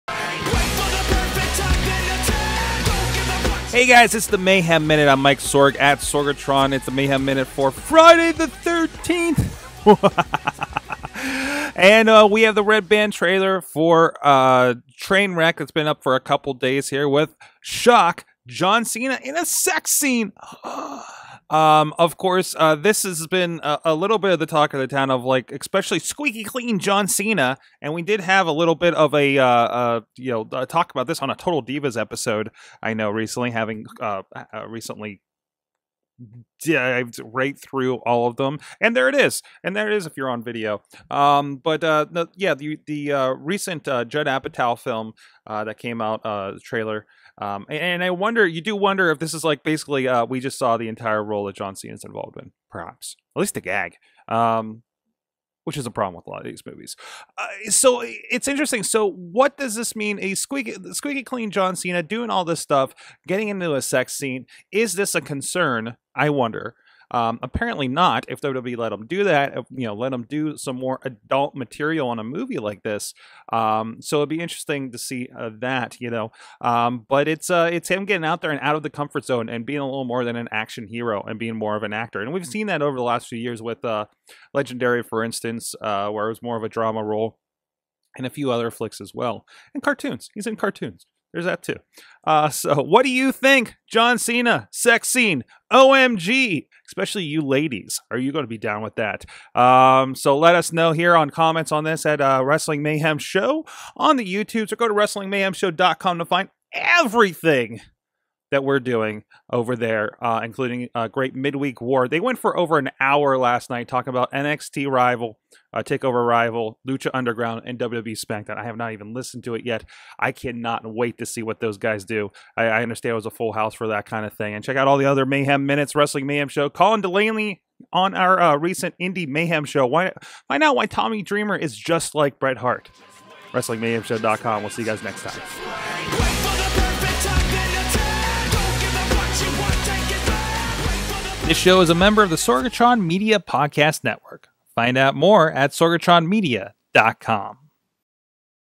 Mayhem Minute: John Cena Sex Scene
John Cena is in the new Judd Apatow film Trainwreck.  In a sex scene???

What do you think of John Cena's sex scene?